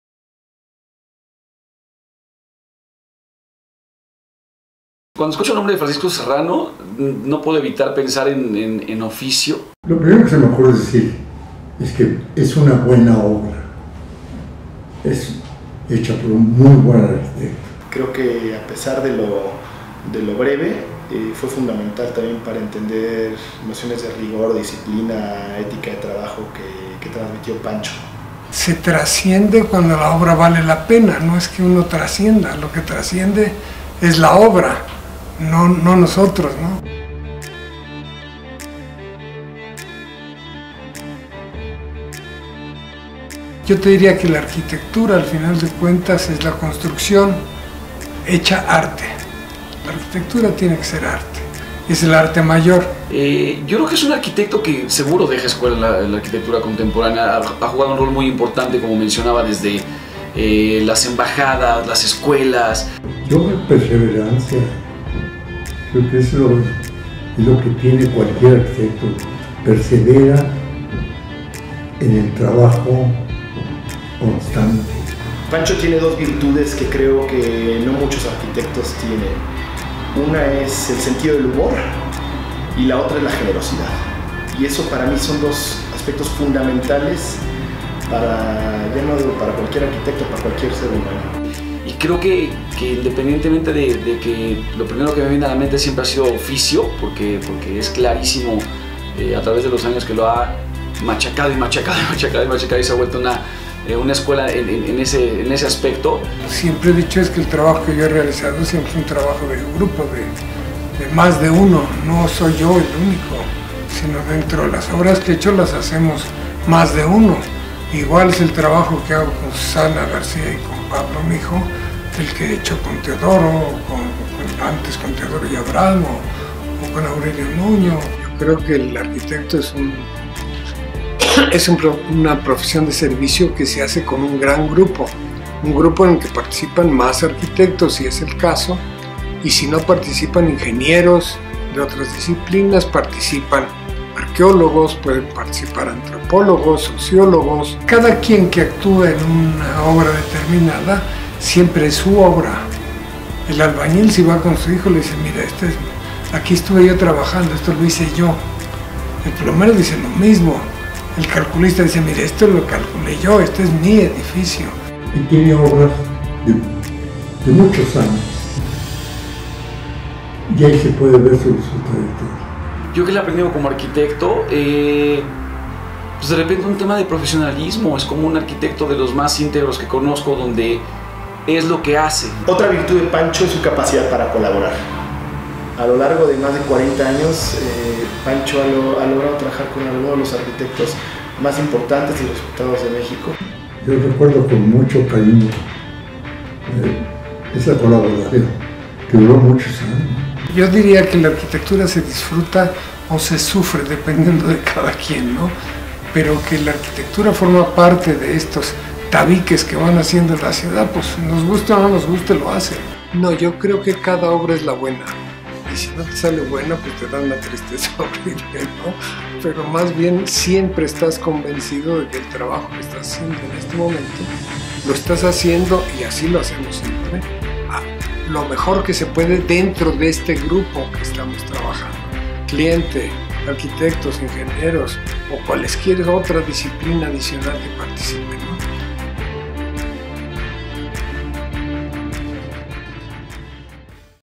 Francisco Serrano, el arquitecto que construye arte
Durante la entrega del Premio Obra del Año de la Revista Obras, el profesionista mexicano fue reconocido por su trayectoria arquitectónica.